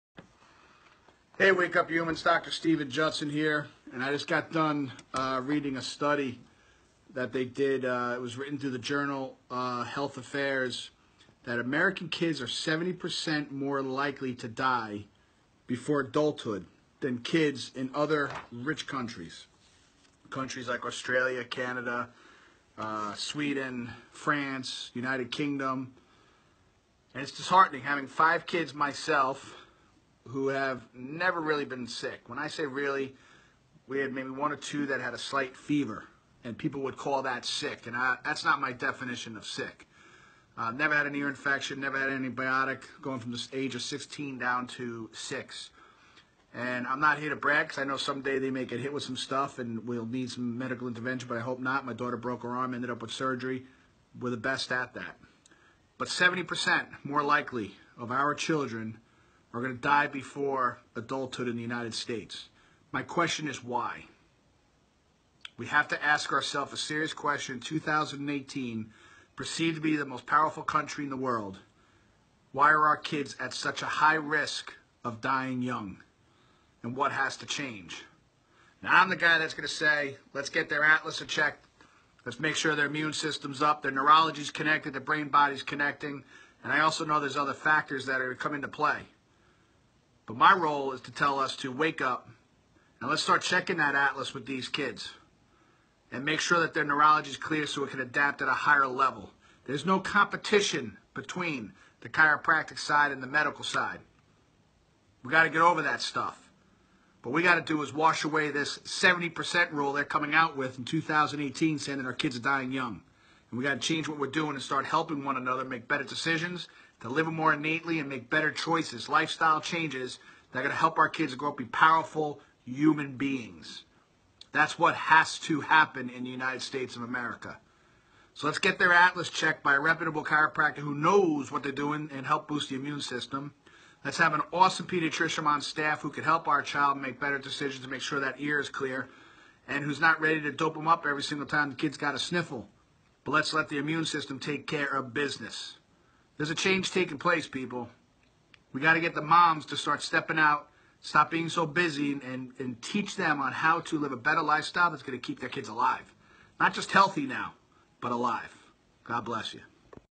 WHAT ARE WE GOING TO DO ABOUT IT?
70% of American kids will die before adulthood than kids in other rich countries... WHAT ARE WE GOING TO DO ABOUT IT?